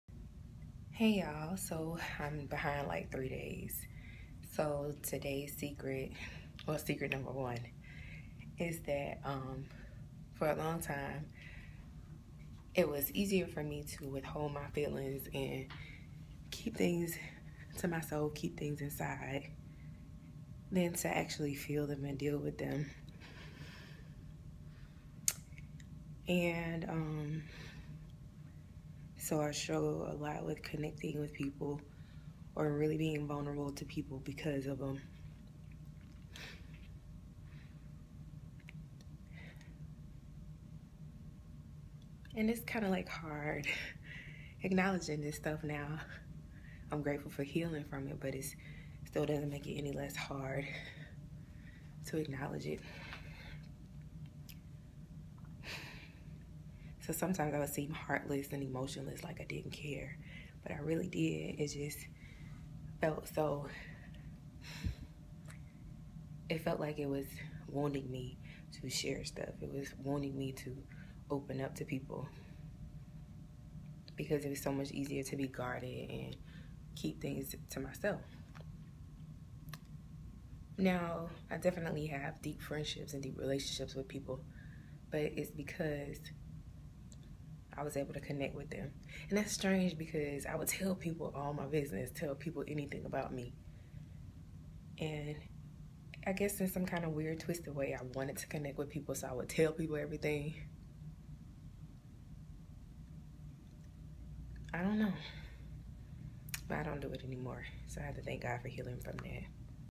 21 Days to Freedom: Day 12 No More Disconnect with My Feelings
At times I didn't know how to feel...how to express my feelings. Sometimes I couldn't connect emotionally because it made me feel vulnerable and open to being attacked. Being heartless and emotionless made life safer to live.